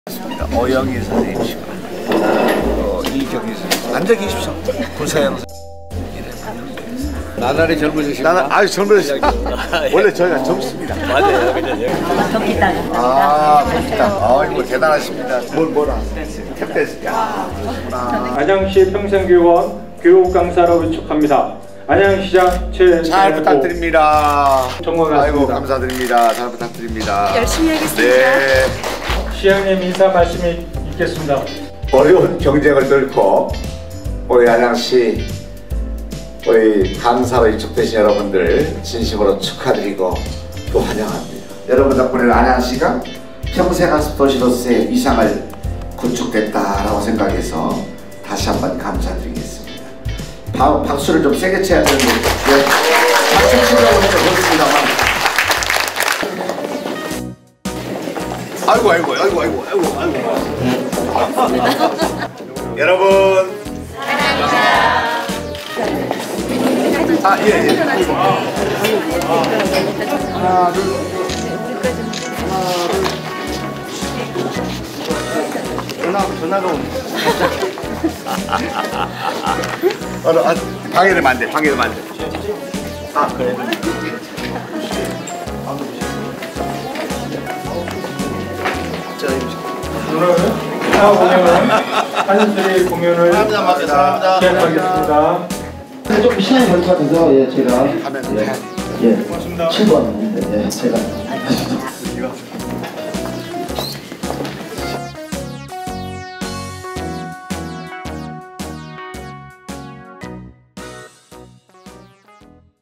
평생교육 우리가 책임진다!/평생교육원 교육강사 위촉식
안양시의 평생교육을 책임지는 평생교육원!

12월 17일 평생교육원에서 교육을 책임지실 강사님들을 만나 위촉장을 전달하기 위해 대호씨가 참석하였습니다.

강사님들을 만나서 여러 이야기도 나누고 같이 사진도 찍는 현장 함께 보시죠~